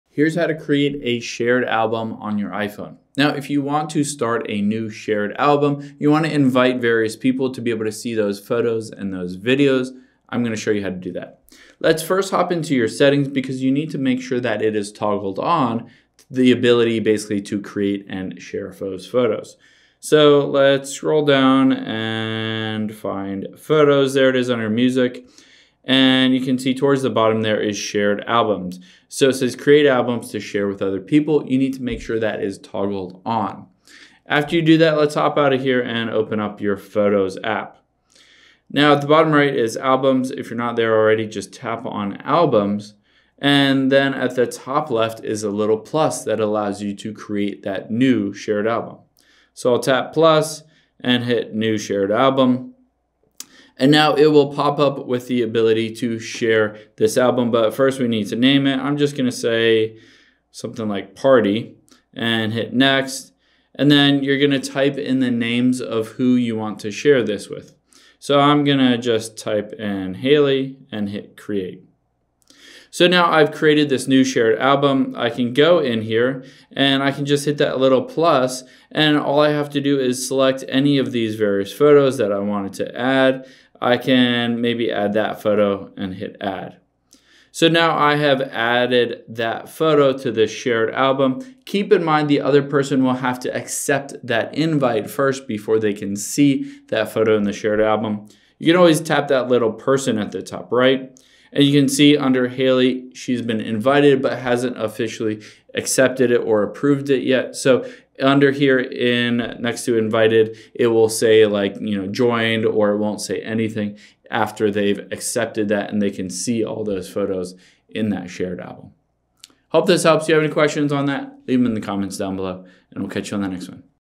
How To Create Shared Album On iPhone
Let's create a shared album on your iPhone so you can share photos and videos with other people and have them contribute photos as well.